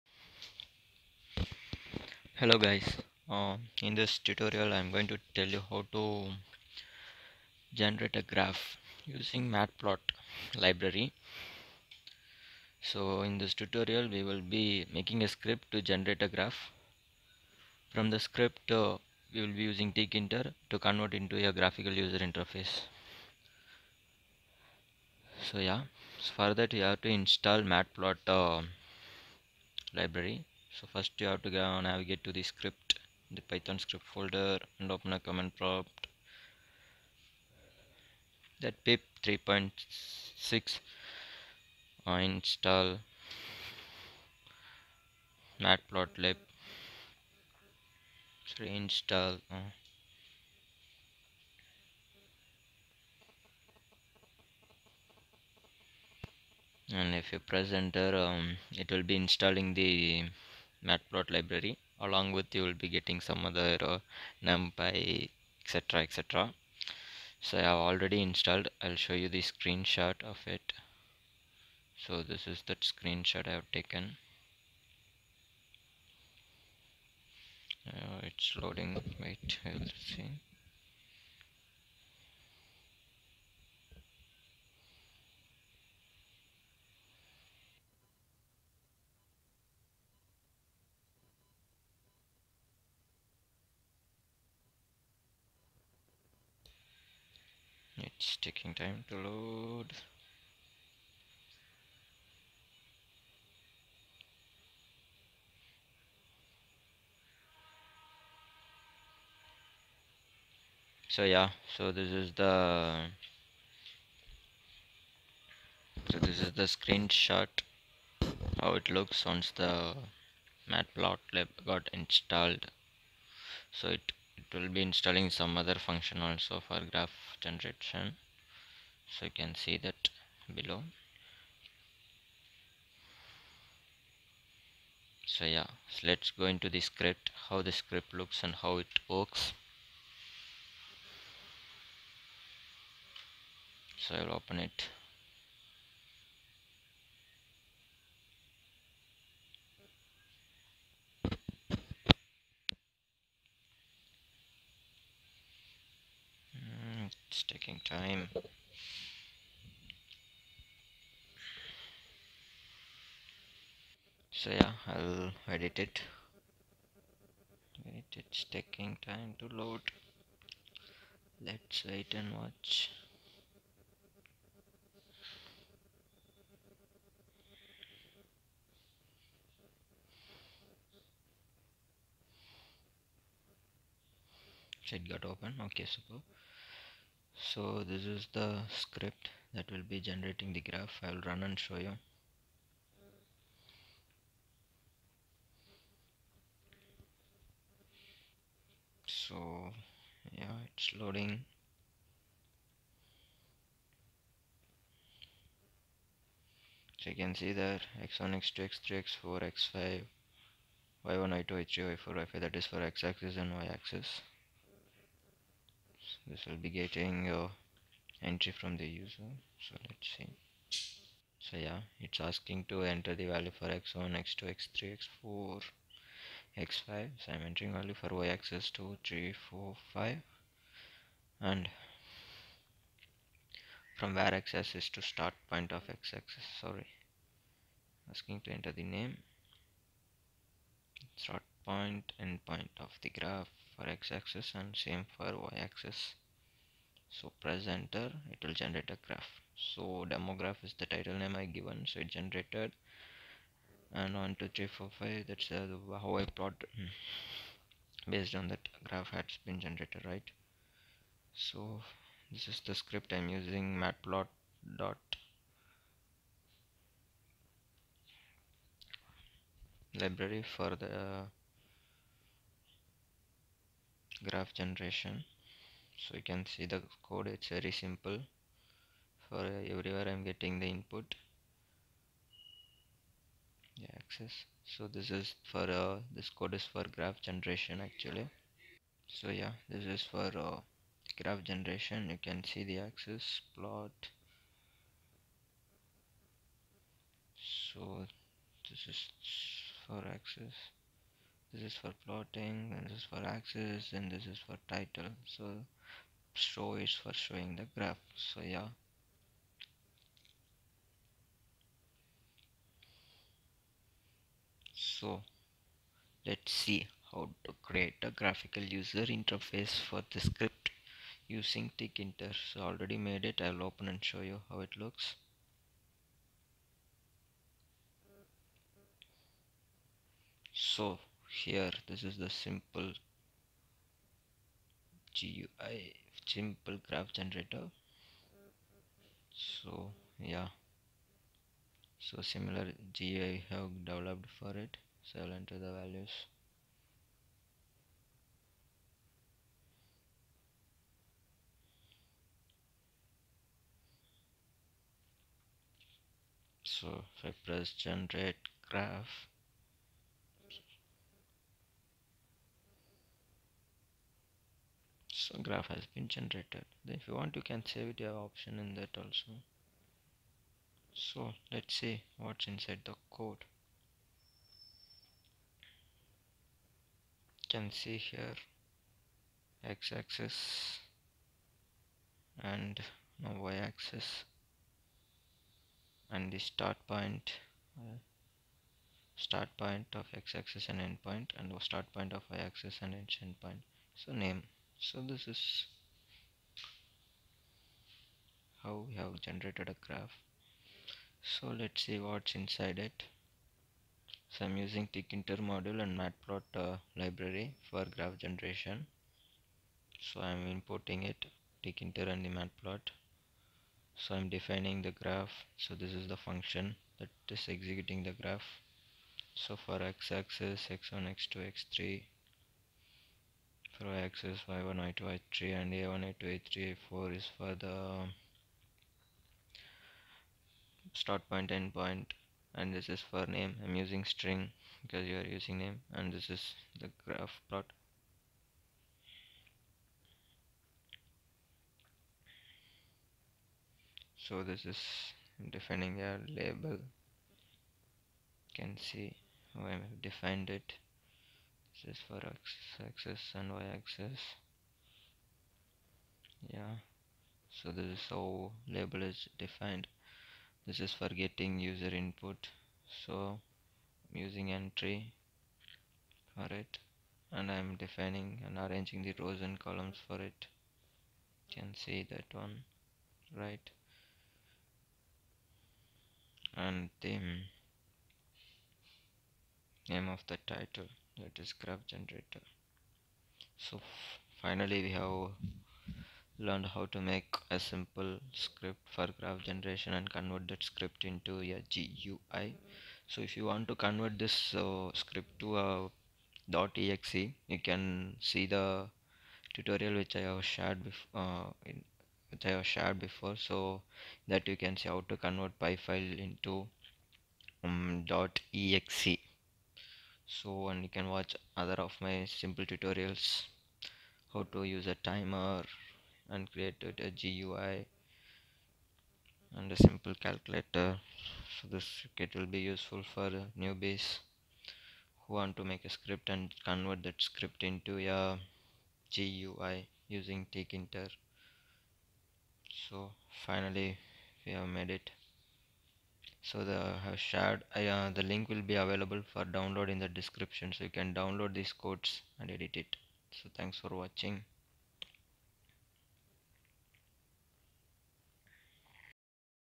How to make graph generator in python from script to GUI using python3.6.2 for newbies
This video for beginners, to learn how to install matpilotlib module in python3.6.2 and using it how to make simple graph generator & develop the script in to GUI using tkinter module.
Python version used is 3.6.2.
Below link for downloading the python files used in this video.